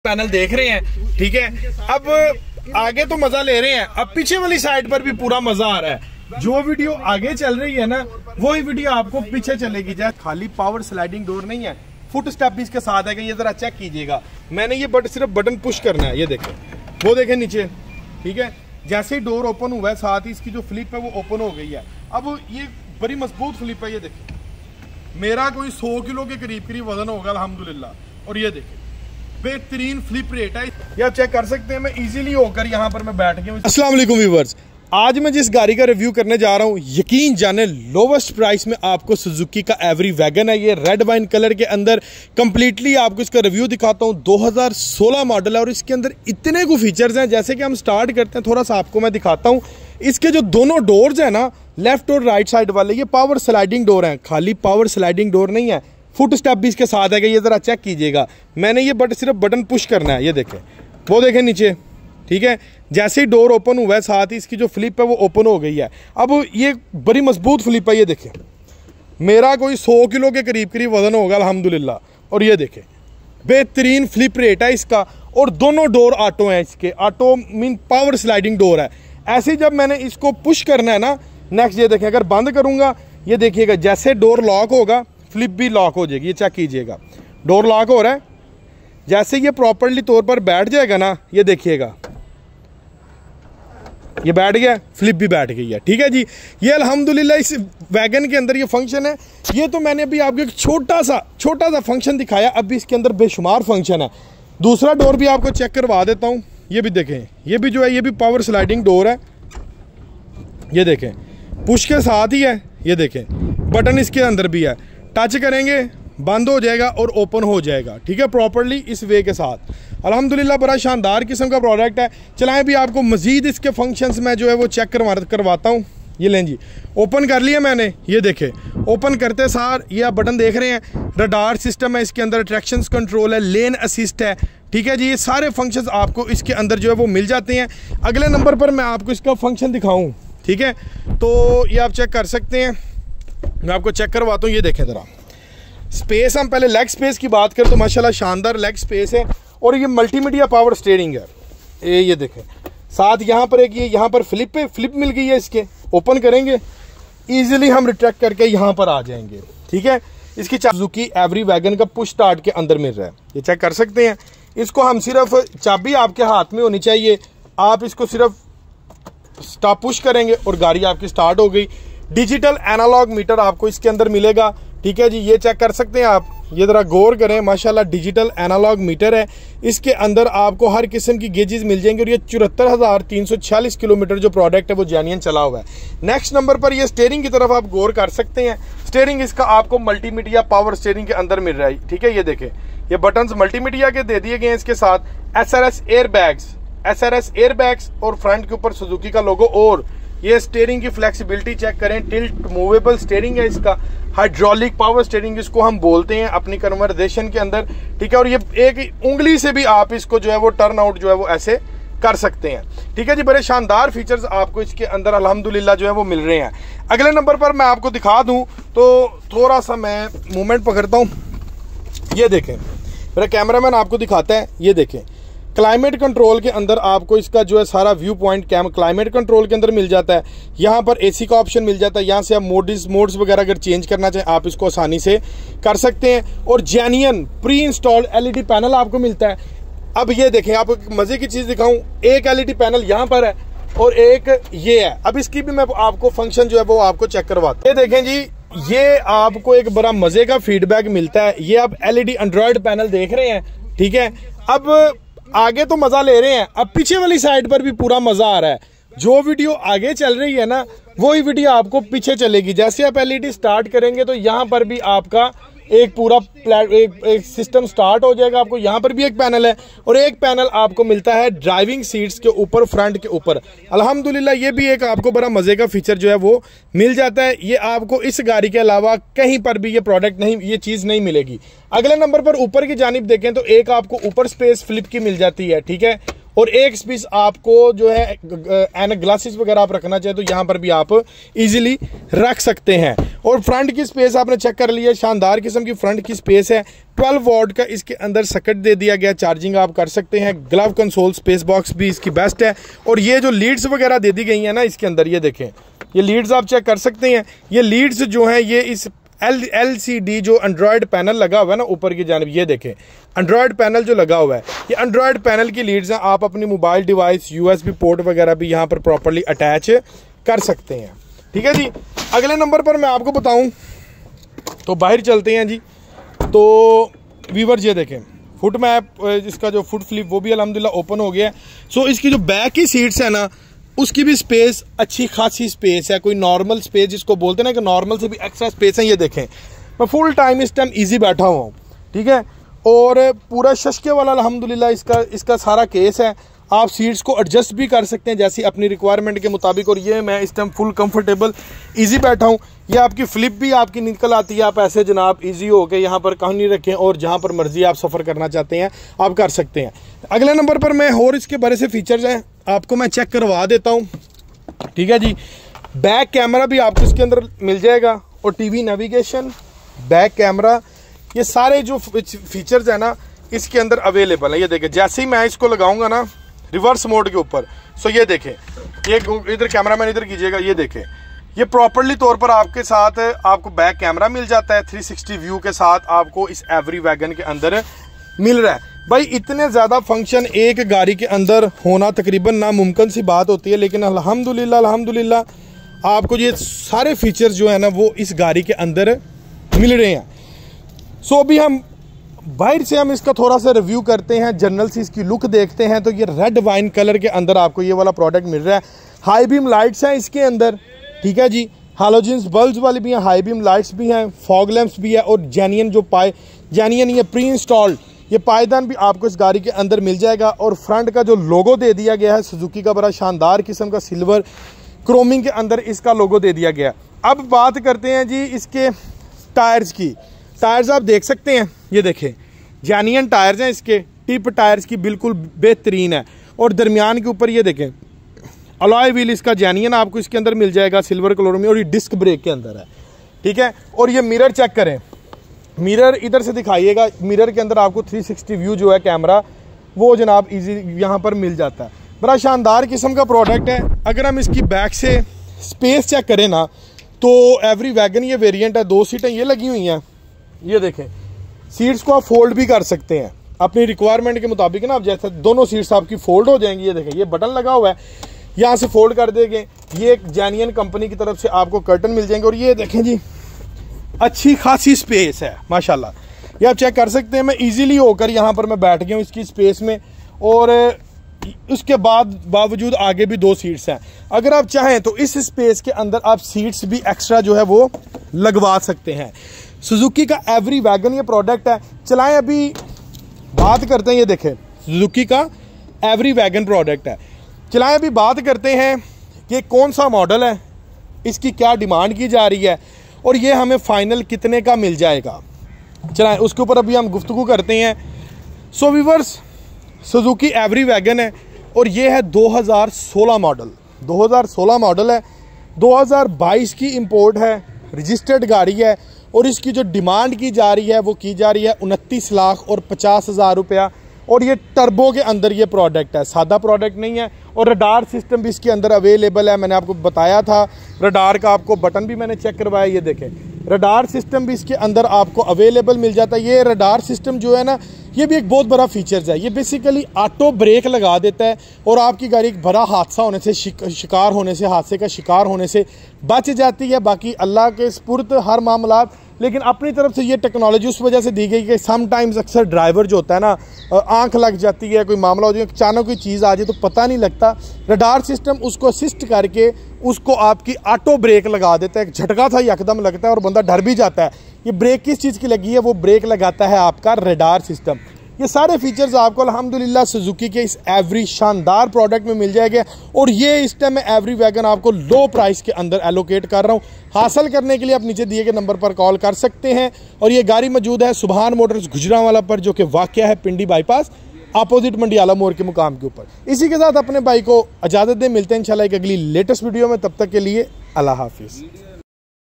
Japanese Low price 660cc Suzuki Every Wagon 2016 Model 2022 import Features is More Than Fortuner
Suzuki Every Wagon Review